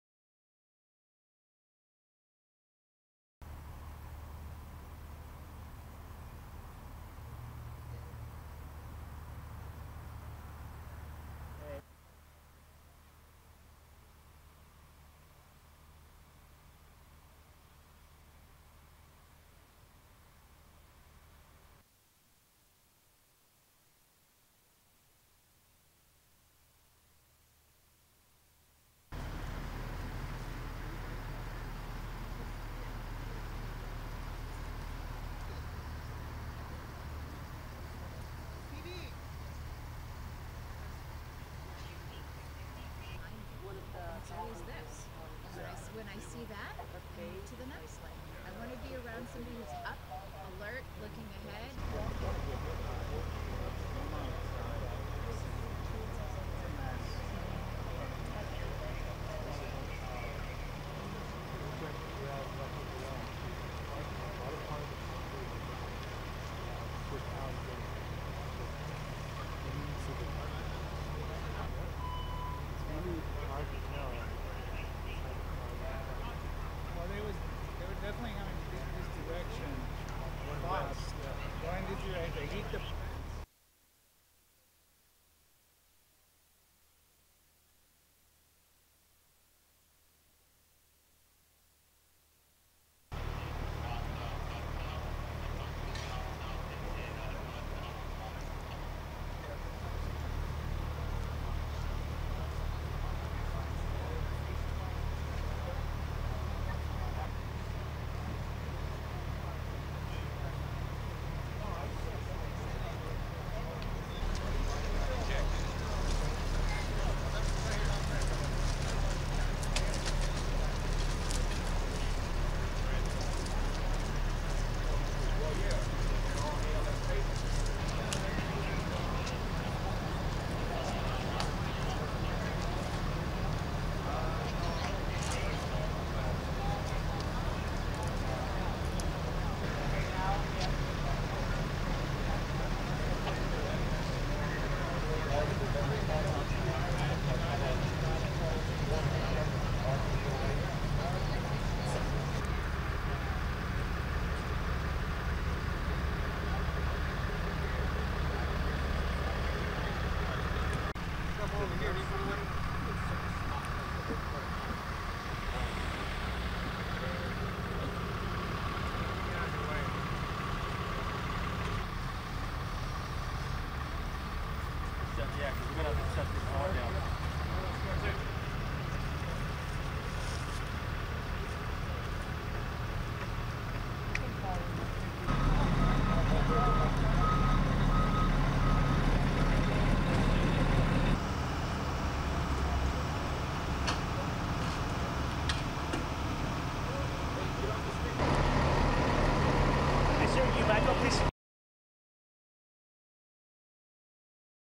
Three Vehicle Collision with One Fatal / West Hollywood RAW FOOTAGE
WEST HOLLYWOOD - One person was killed and three others sustained critical injuries in a major crash between at least three vehicles including a parked BMW and a minivan. The first vehicle struck the two at a very high rate of speed, overturning and possibly ejecting someone into the street. The three surviving victims were transported to local hospitals and an investigation is in progress with surrounding streets closed for unknown duration.